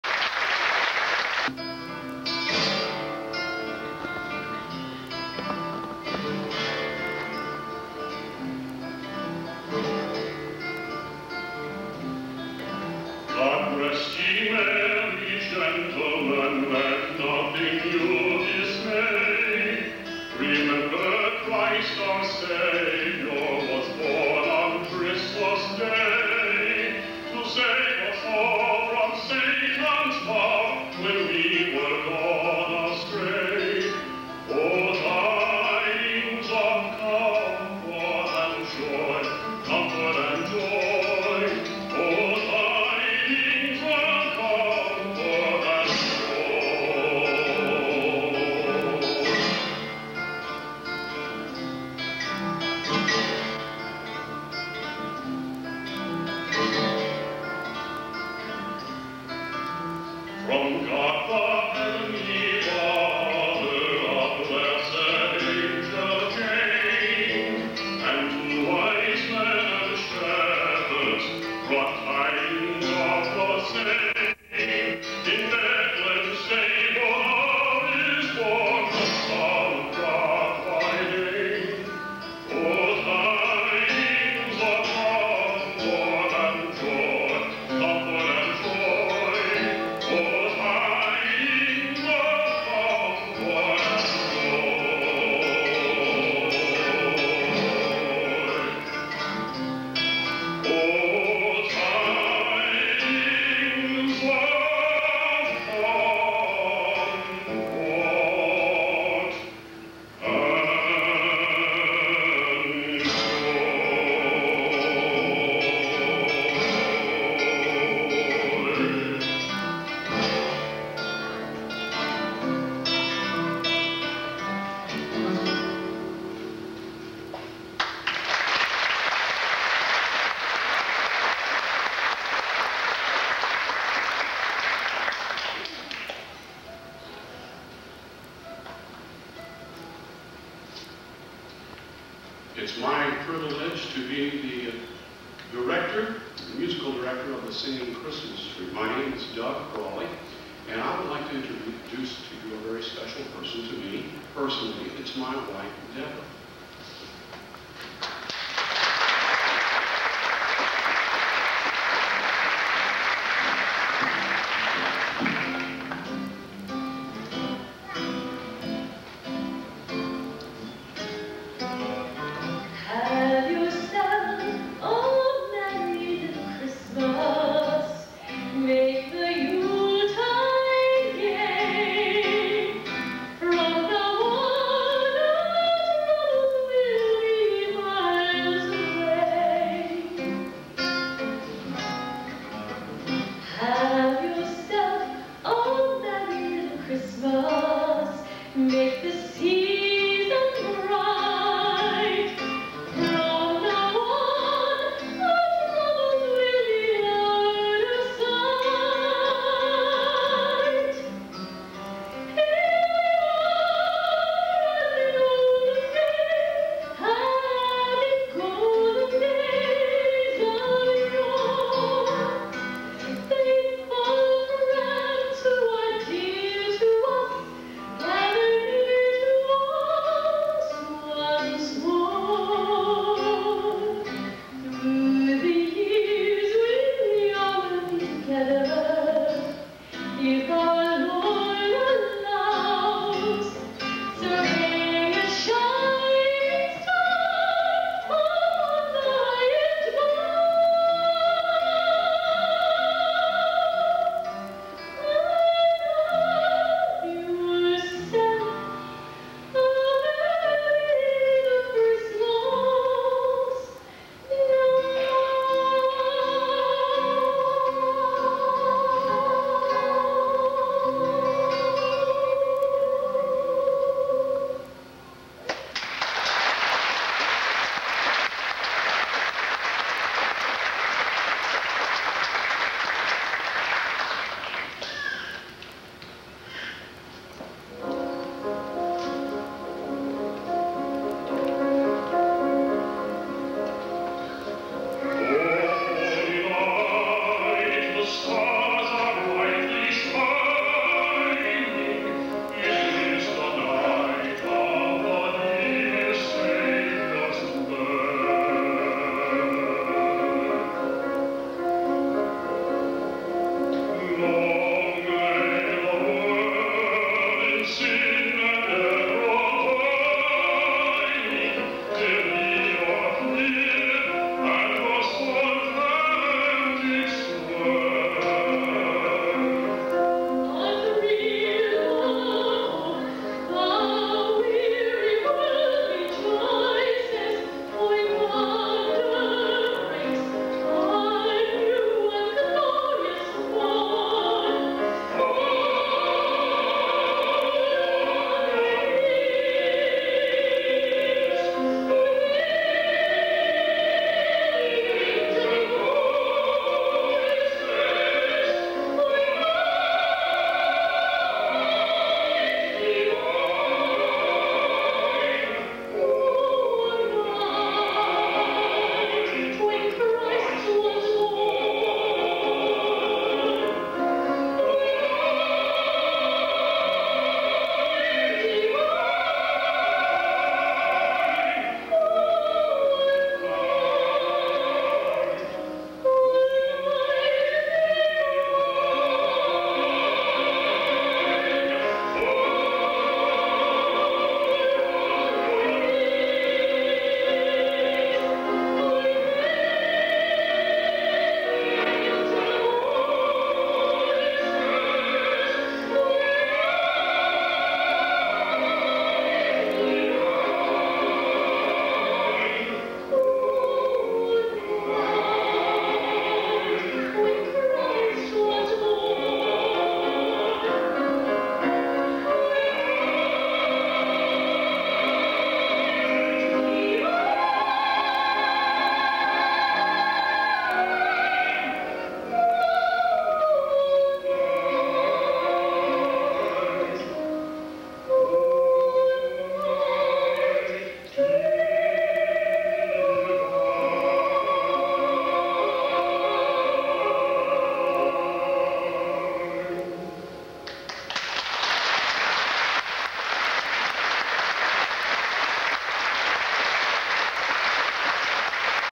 DOUG & DEBRA CRAWLEY, BRIGHTON ENGLAND SD 480p
Doug and Debra Crawley singers; Debbie Drake, Piano; Far Hills Singing Christmas Tree; Brighton Dome Theatre, Brighton, England (1993)